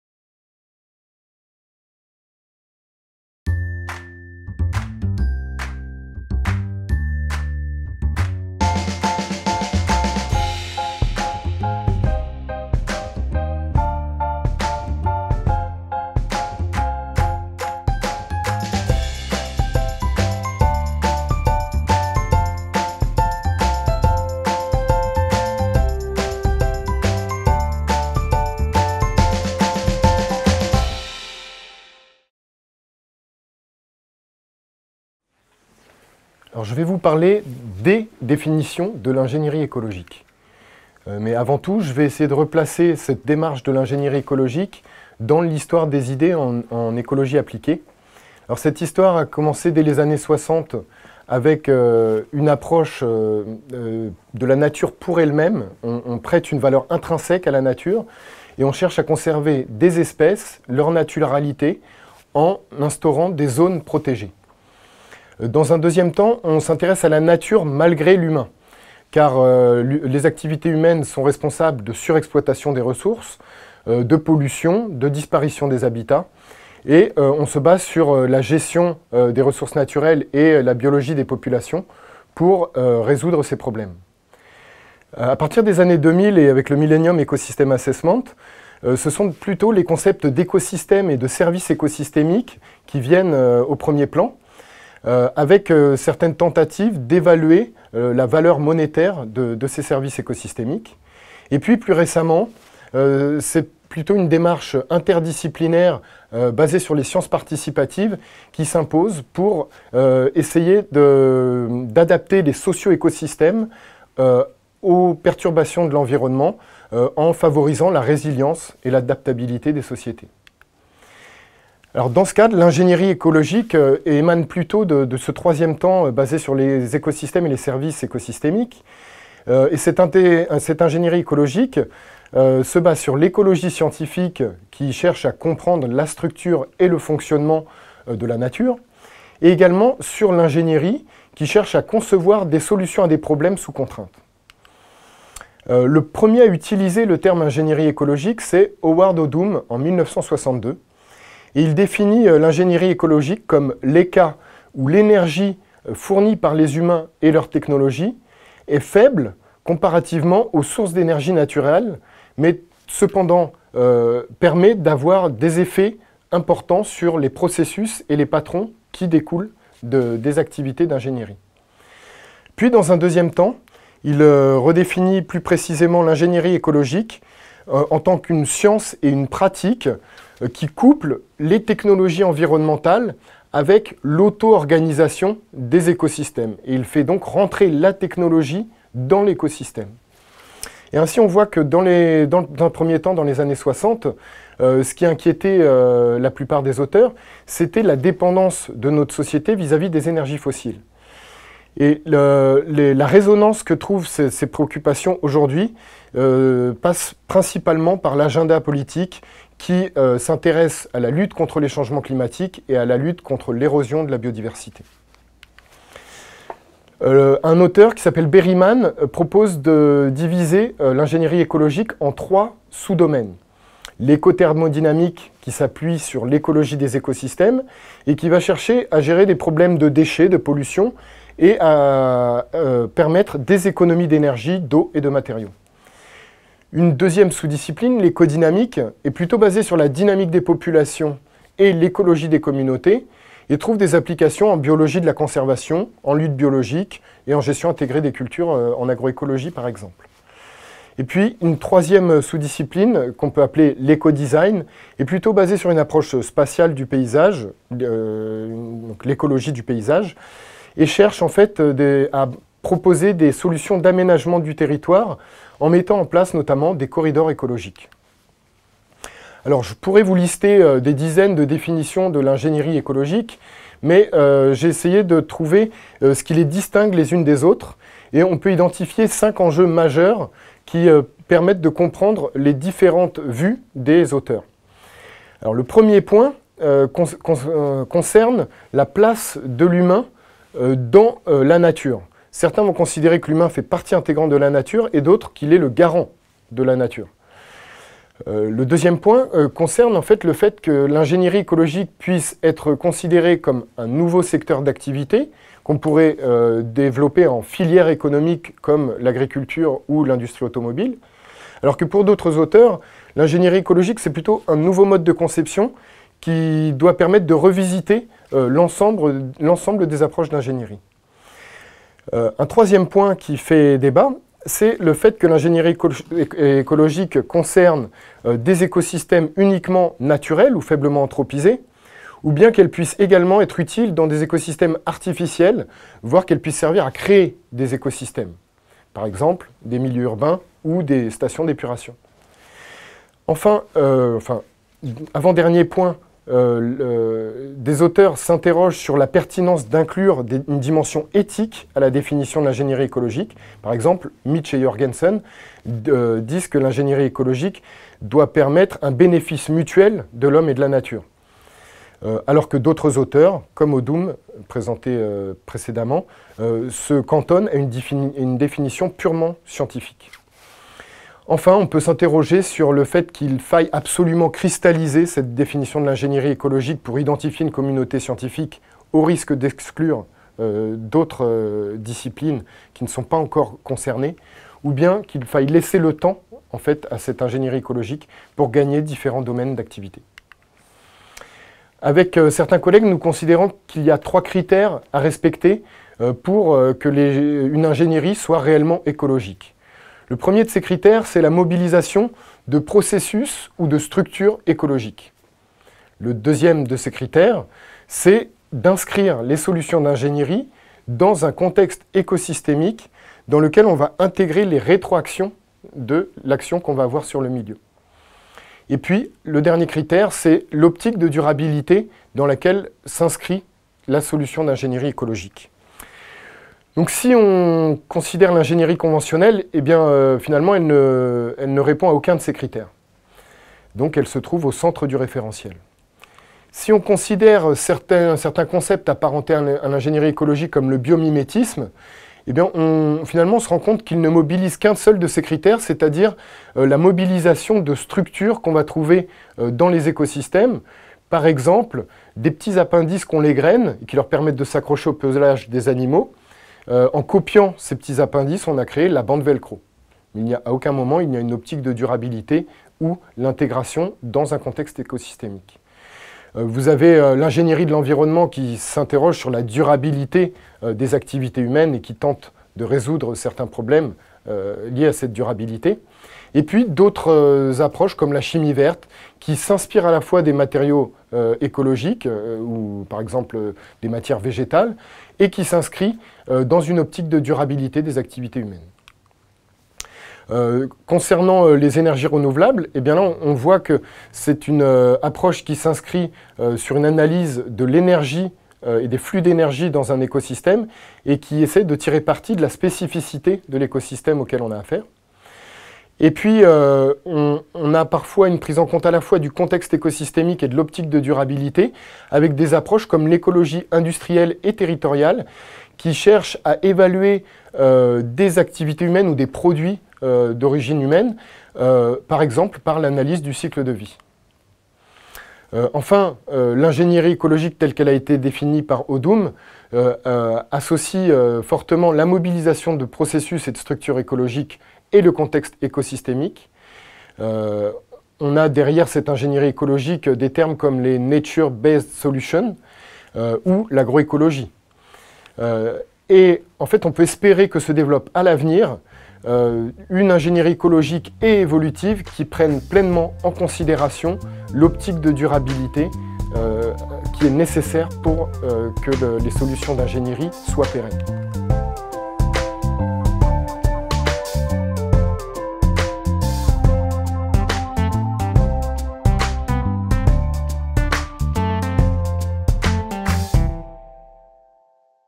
Les définitions de l'ingénierie écologique
Manuel Blouin, professeur à AgroSup Dijon, discute dans cette vidéo (9'35) des définitions qui ont été faites et qui aujourd'hui pourraient être proposées de l'ingénierie écologique. Au-delà d'un inventaire des définitions et de leurs auteurs, il met en évidence une grille de critères et d'enjeux qui lui paraissent pertinents pour tenter de définir les contours de cette ingénierie écologique.